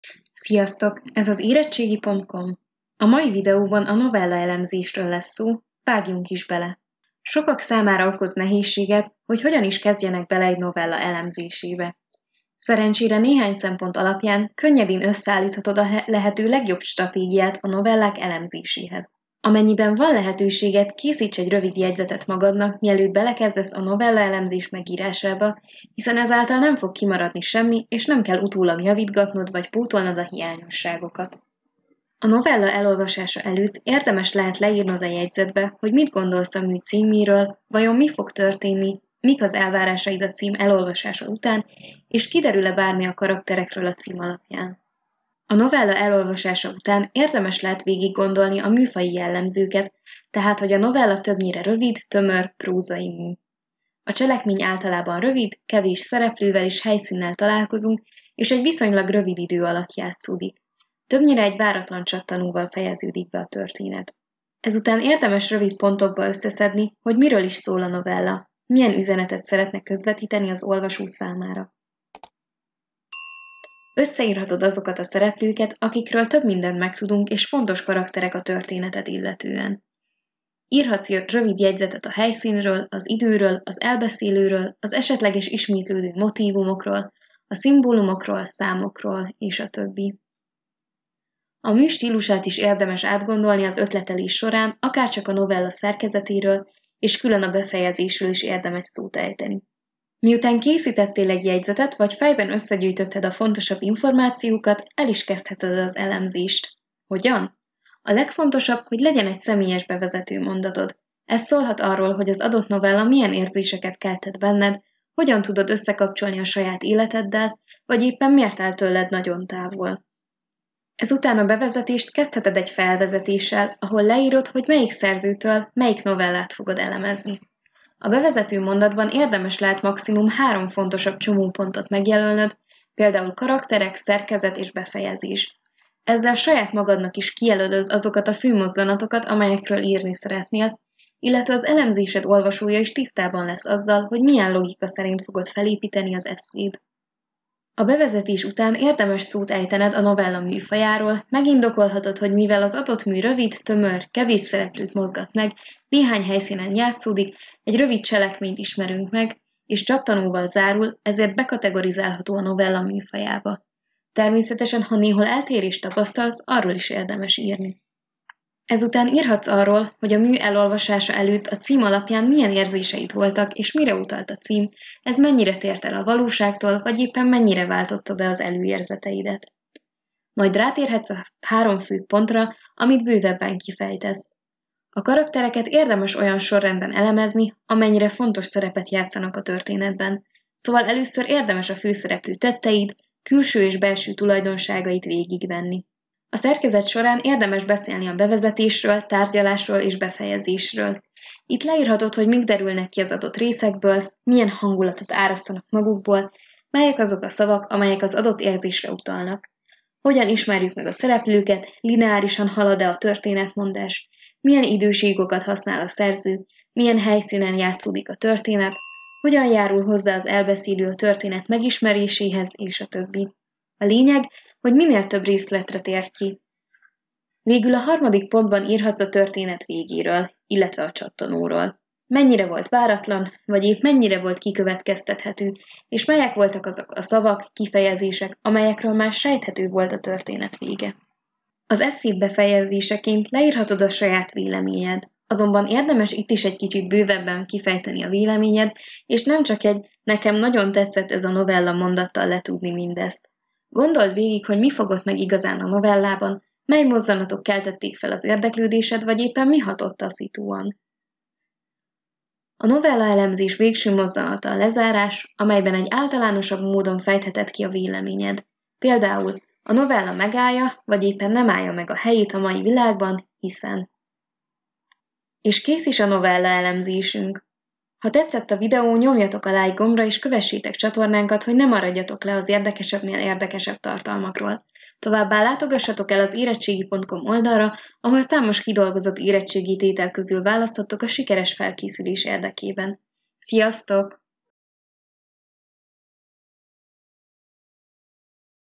Novellaelemzés hogyan? - Irodalom érettségi tétel | Erettsegi.com
Segítünk felkészülni a novella elemzésből. Hogyan kezdj neki, mi az amit mindenképpen tartalmazzon...